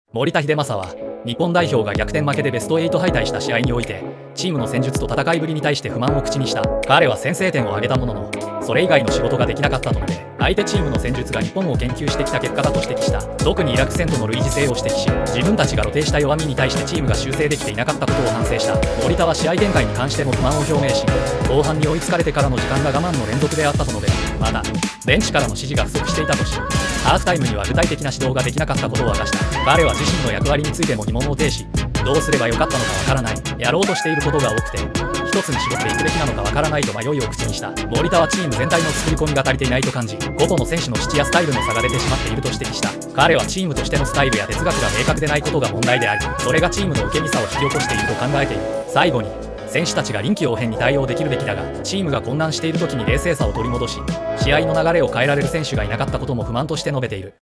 苦悩を吐露した守田英正の悲痛な叫び
守田英正は、日本代表が逆転負けでベスト8敗退した試合において、チームの戦術と戦いぶりに対して不満を口にした。